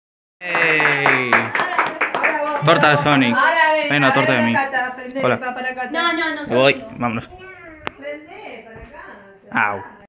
La torta Sonic
es el cumple de mi hermanito menor :D